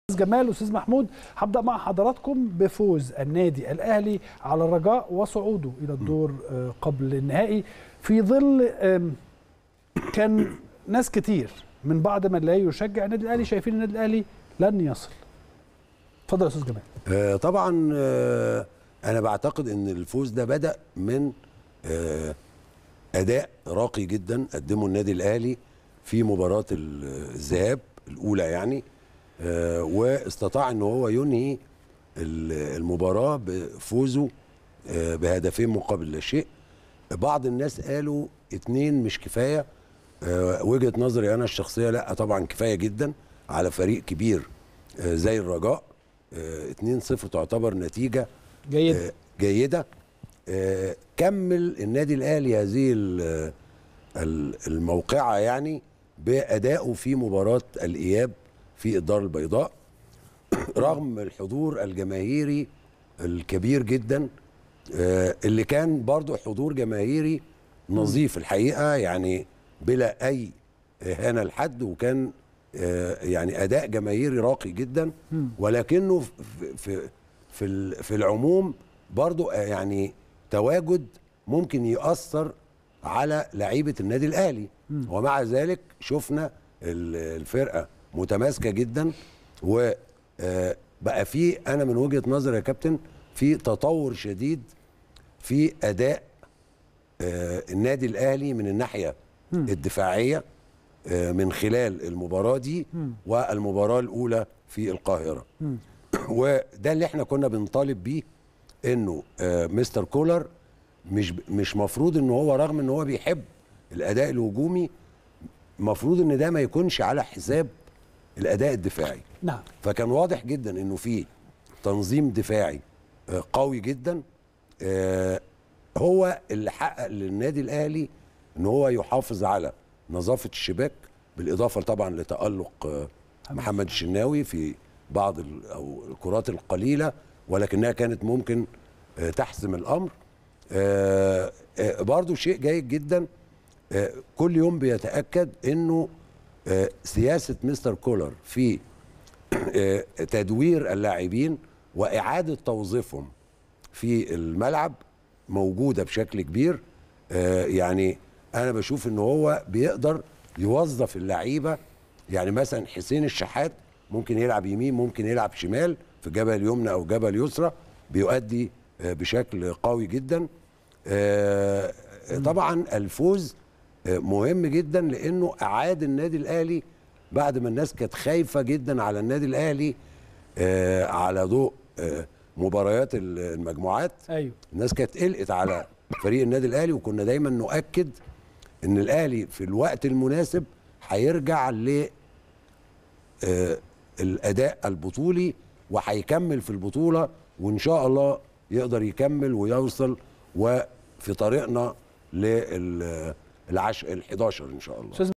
المرونة التكتيكية وسياسة التدوير.. حوار - جمال الزهيري ورأيه في تأهل الأهلي لنصف نهائي الأبطال
المرونة التكتيكية وسياسة التدوير.. حوار - جمال الزهيري ورأيه في تأهل الأهلي لنصف نهائي دوري الأبطال علي حساب الرجاء المغربي